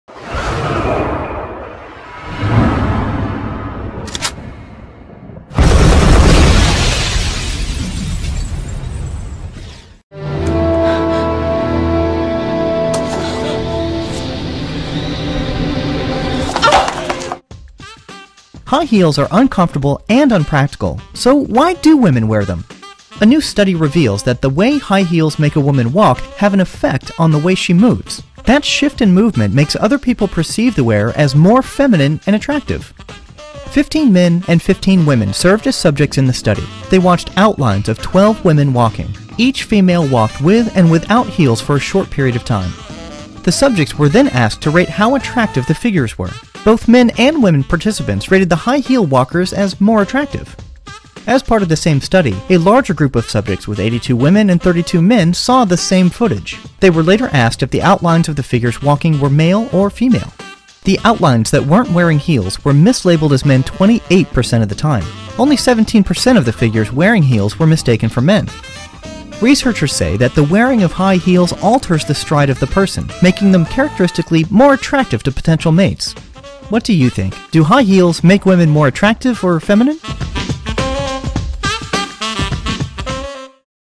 New Study Explains Why Women Wear High Heels
High heels are uncomfortable and unpractical, so why do women wear them? A new study reveals that the way high heels make a woman walk has an effect on the way she moves. That shift in movement makes other people perceive the wearer as more feminine and attractive. 15 men and 15 women served as subjects in the study. They watched outlines of 12 women walking. Each female walked with and without heels on for a short period of time.